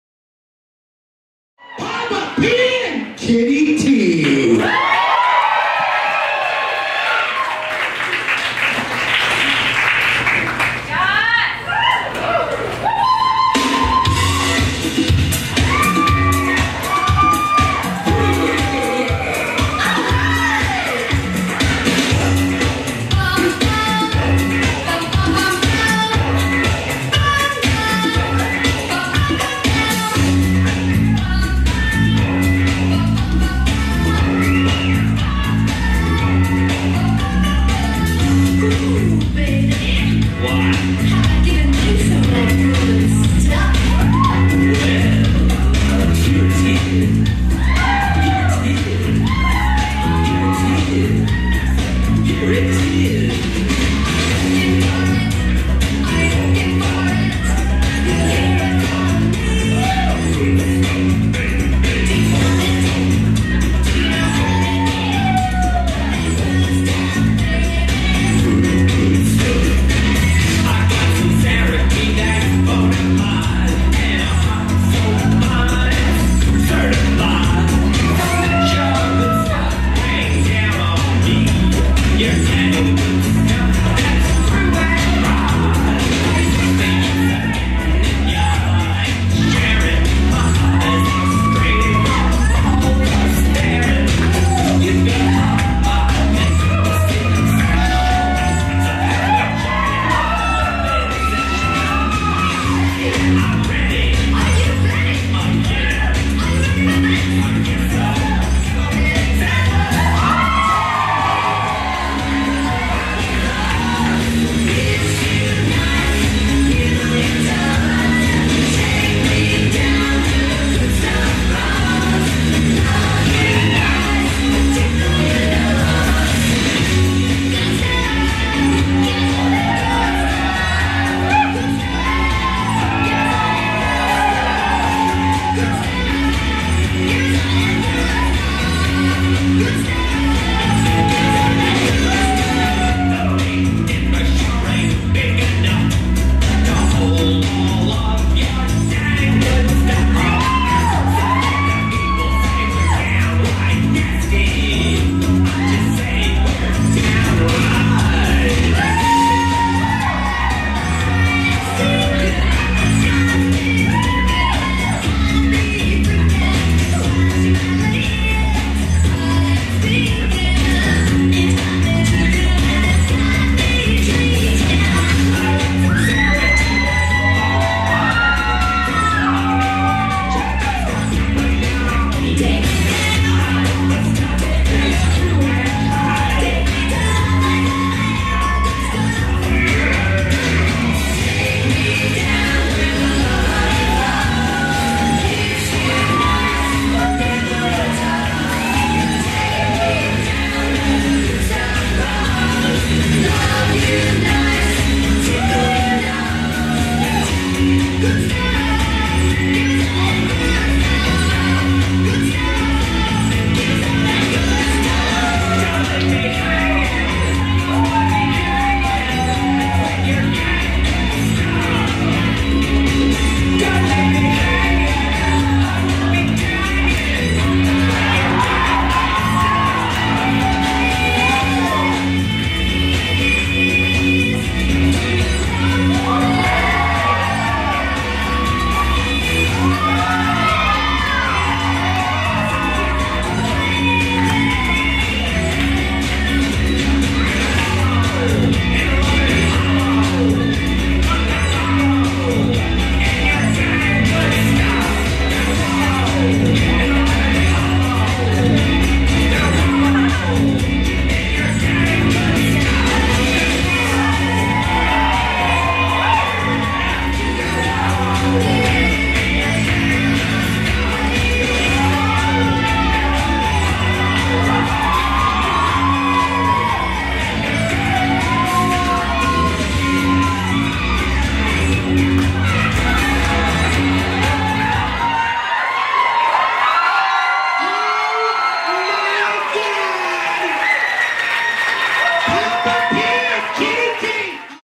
MiBF '17 - Pappa P and Kitty T (Friday)
Pappa P and Kitty Tornado performs at the 2017 Michigan Burlesque Festival, presented by Fancy Pants Arts & Entertainment.
Hosted at the Hastings Street Ballroom, Detroit.
Video and editing by: Subpar Studios